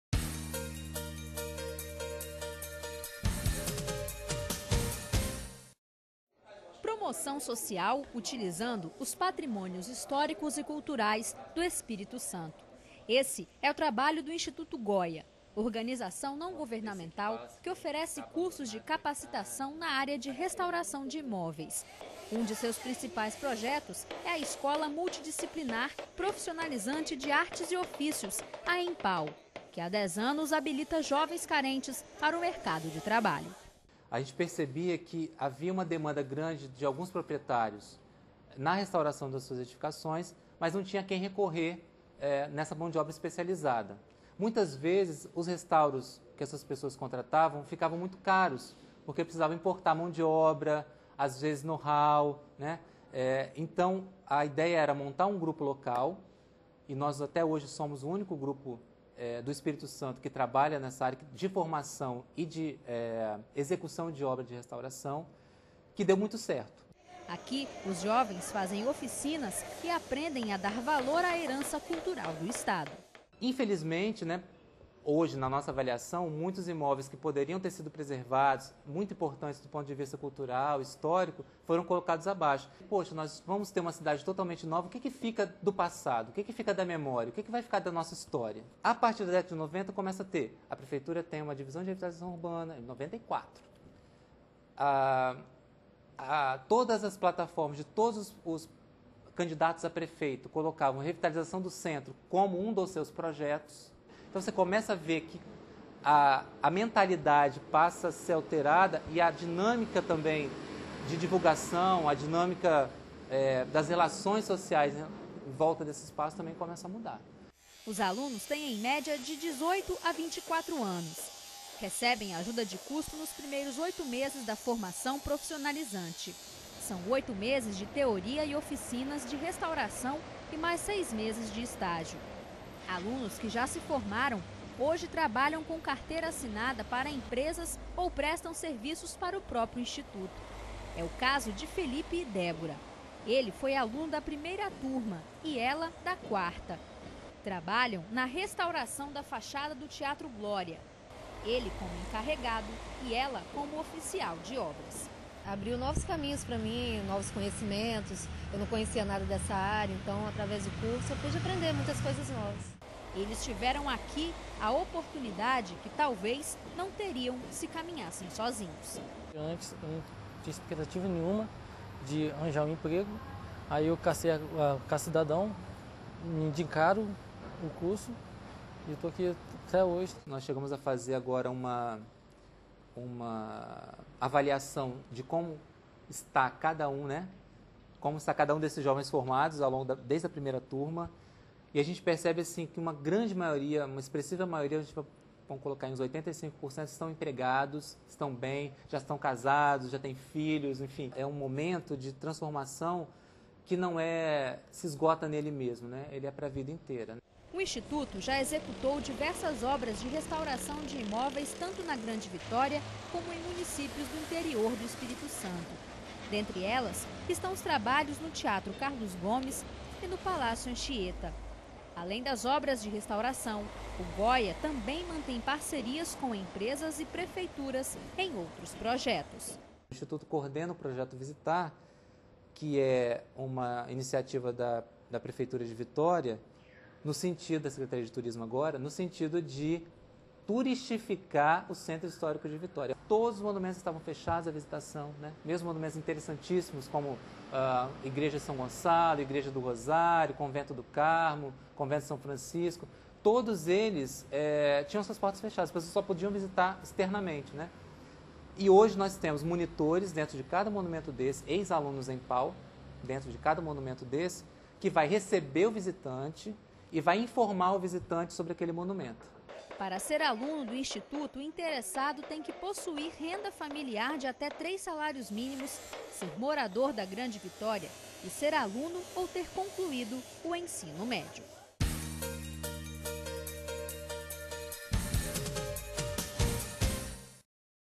ALES COMUNIDADE - INSTITUTO GOYA
Conheça o trabalho realizado pela entidade que ensina aos jovens técnicas de restauração de prédios históricos do Estado.